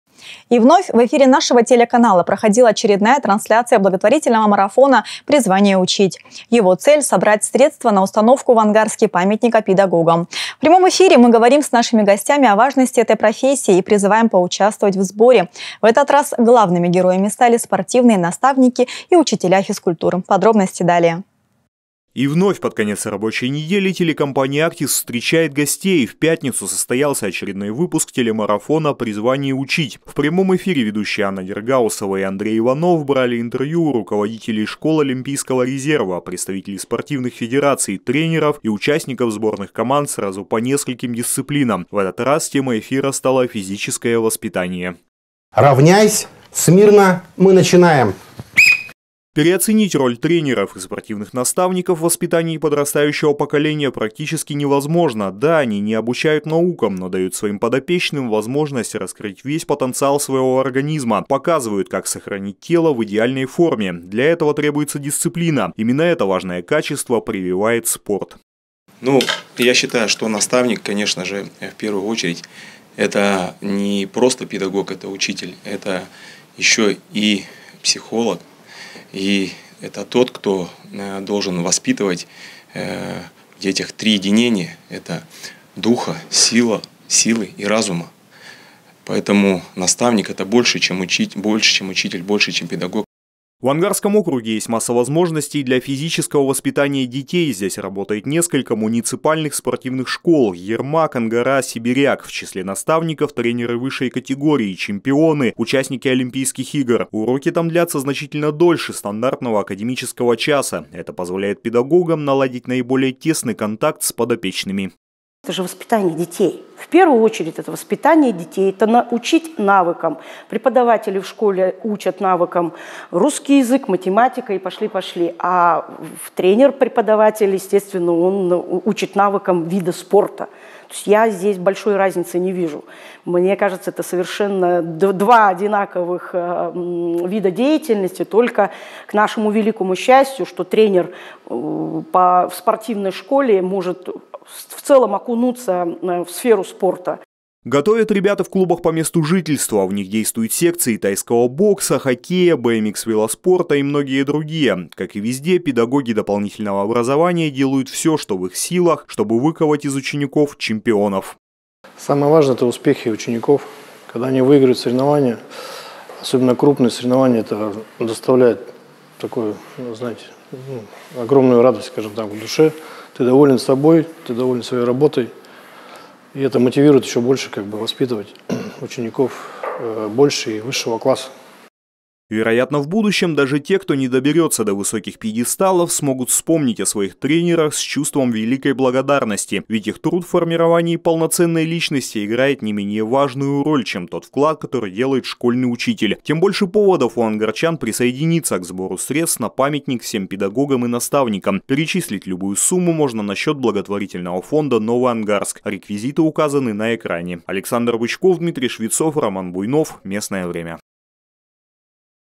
«Больше, чем педагог»
И вновь в эфире нашего телеканала проходила очередная трансляция благотворительного марафона «Призвание - учить». Его цель - собрать средства на установку в Ангарске памятника педагогам. В этот раз главными героями стали спортивные наставники и учителя физкультуры.